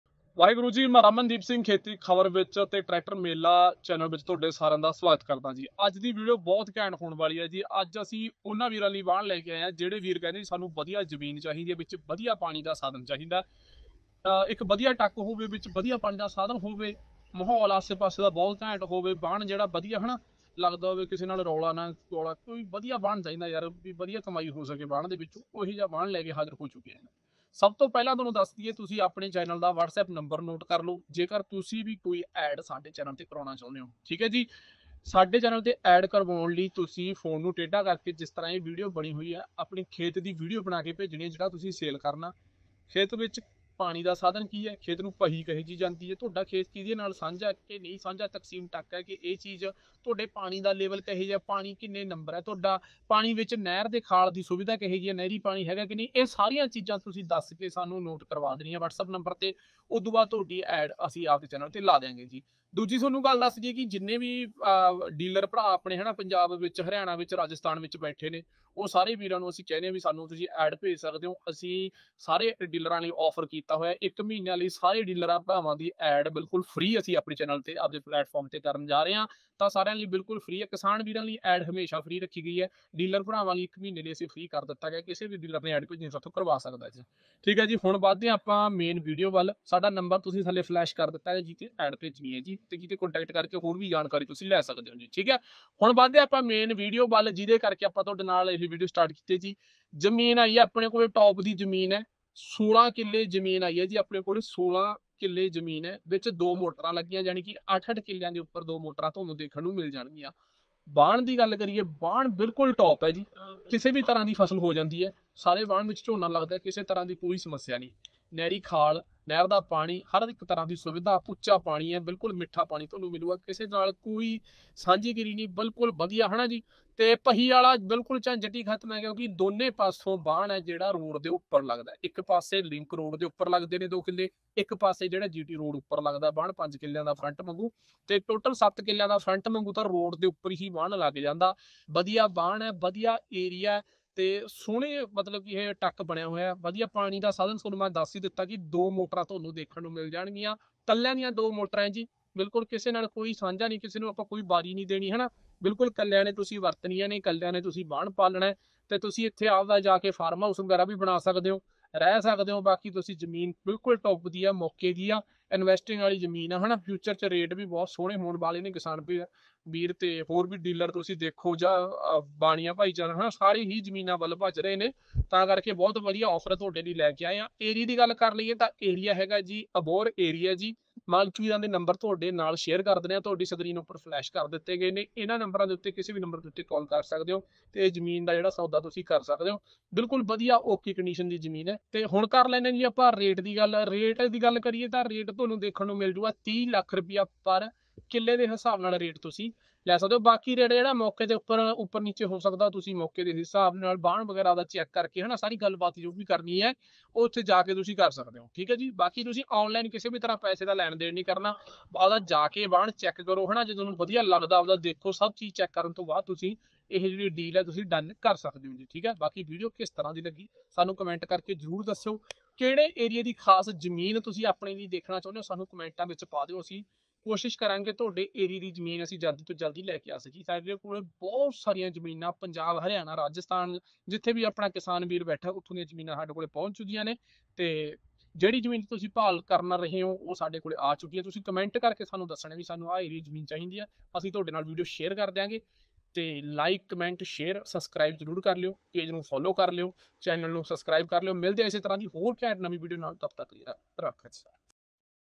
16 ਕਿੱਲੇ ਜਮੀਨ ਵਿਕਾਊ ਵਿੱਚ 2 ਮੋਟਰਾਂ ਟੋਪ ਦੀ ਜਮੀਨ ਆ ਬਹੁਤ ਹੀ ਵਧੀਆ ਰੇਟ ਦੇ ਵਿੱਚ ( Punjab)
"ਟਰੈਕਟਰ ਮੰਡੀ"
"ਪੁਰਾਣਾ ਟਰੈਕਟਰ ਮਾਰਕੀਟ"
,ਸੋਨਾਲੀਕਾ,
ਫੋਰਡ,
ਫਾਰਮਟੇਕ,
ਮੈਸੀ ਪ੍ਰੀਤ,
ਸਵਰਾਜ 855,
ਮੰਡੀ,

16 ਕਿੱਲੇ ਜਮੀਨ ਵਿਕਾਊ ਵਿੱਚ 2 ਮੋਟਰਾਂ ਟੋਪ ਦੀ ਜਮੀਨ ਆ ਬਹੁਤ ਹੀ ਵਧੀਆ ਰੇਟ ਦੇ ਵਿੱਚ ( Punjab)